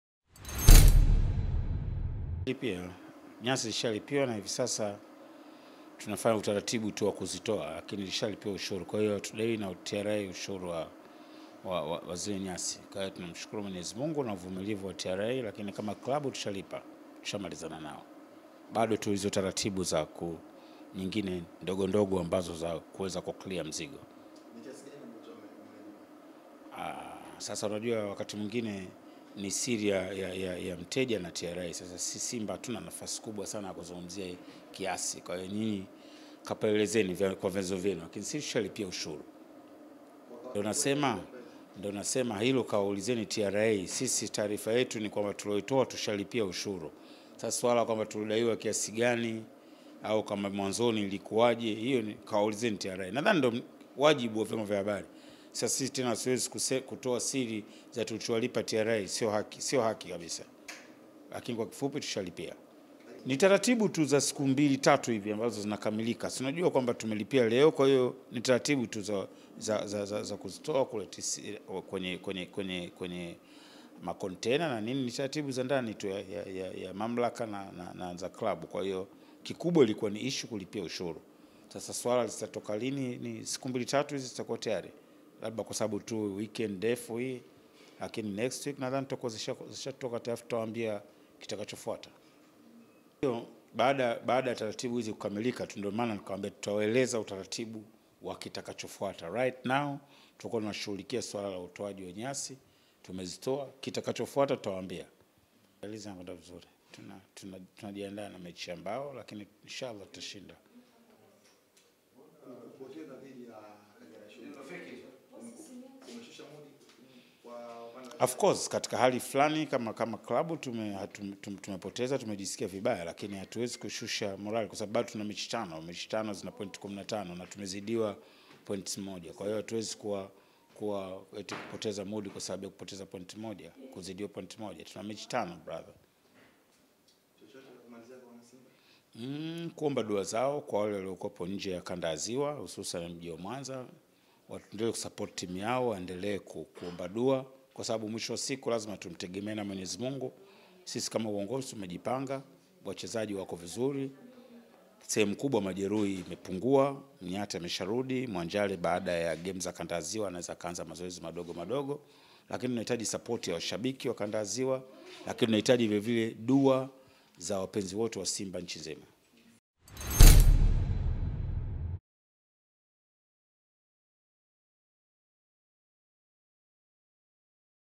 Maamuzi waliyofikia Simba kuhusu nyasi zao bandia bandarini
Moja kati ya stori kubwa iliyokuwa ikijadiliwa na wapenda soka bongo March 30 ni kuhusiana na habari za nyasi bandia za timu ya Simba kuandikwa kuwa zitapigwa mnada na mamlaka ya mapato Tanzania (TRA) kutokana na kushindwa kulilipiwa kodi bandarini lakini leo April 6 2017 wamefanikiwa kulipia kodi hiyo